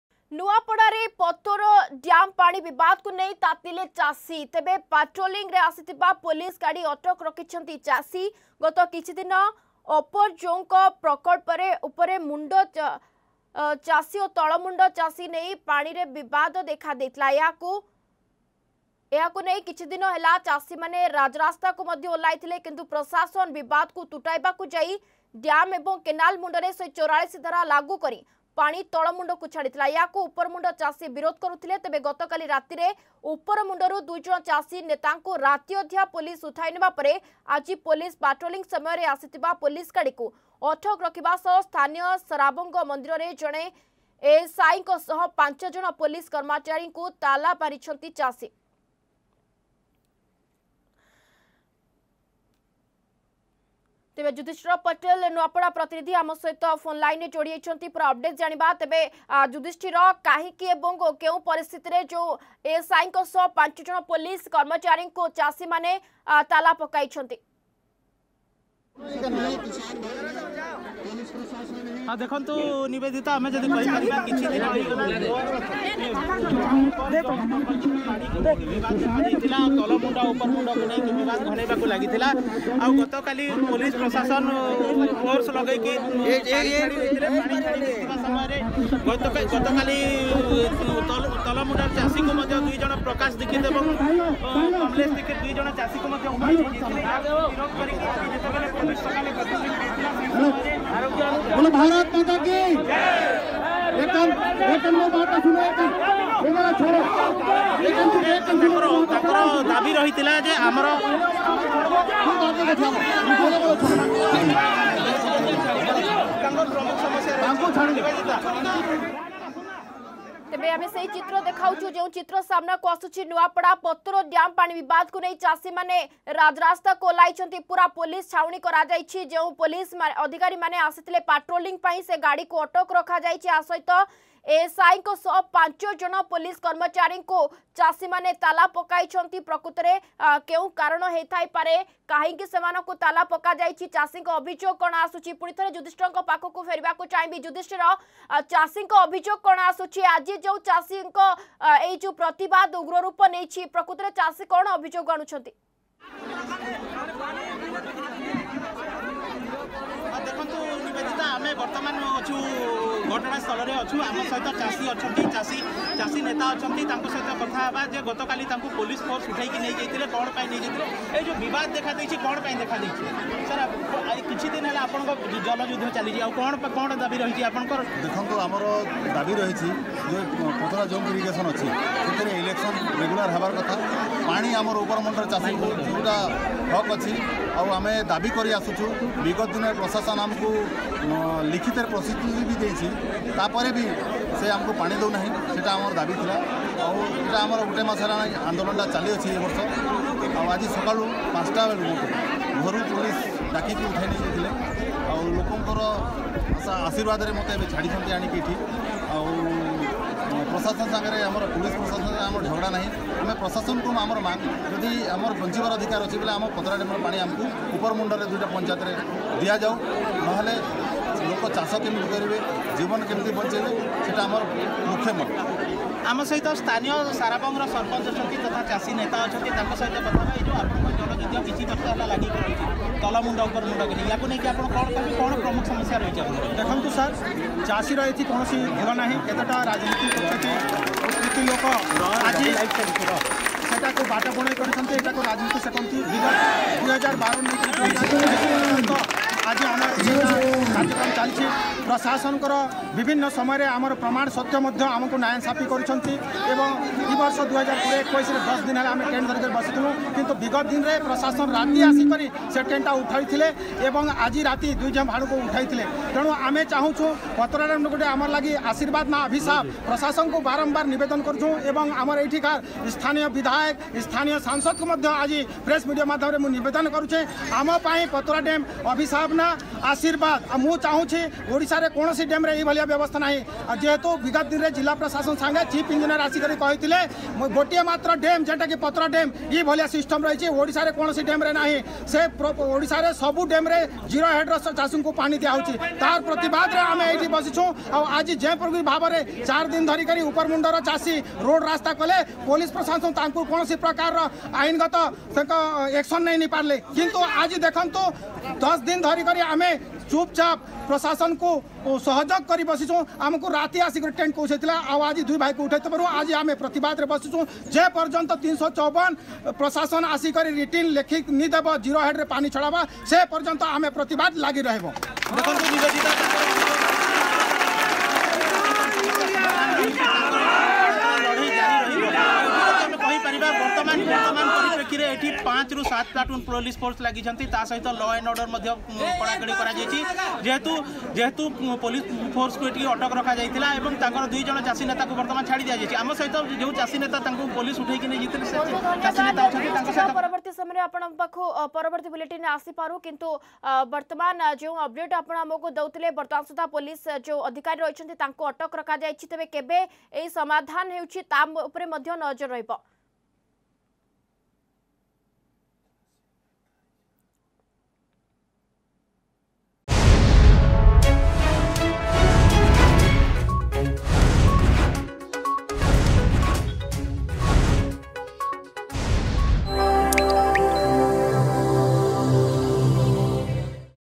ନୂଆପଡ଼ାରେ ପତୋର ଡ୍ୟାମ ପାଣି ବିବାଦକୁ ନେଇ ତାତିଲେ ଚାଷୀ
ନୂଆପଡ଼ାରେ ପତୋର ଡ୍ୟାମ ପାଣି ବିବାଦକୁ ନେଇ ତାତିଲେ ଚାଷୀ । ପାଟ୍ରୋଲିଂରେ ଆସିଥିବା ପୋଲିସ ଗାଡ଼ି ଅଟକ ରଖିଛନ୍ତି ଚାଷୀ । ଗତ କିଛି ଦିନ ଅପର ଜୋଙ୍କ ପ୍ରକଳ୍ପରେ ଉପର ମୁଣ୍ଡ ଚାଷୀ ଓ ତଳ ମୁଣ୍ଡ ଚାଷୀଙ୍କୁ ନେଇ ପାଣି ବିବାଦ ଦେଖାଦେଇଥିଲା.
ଅଧିକ ଖବର ପାଇଁ ଡ଼ାଉନଲୋଡ କରନ୍ତୁ “Argus News” ଆପ୍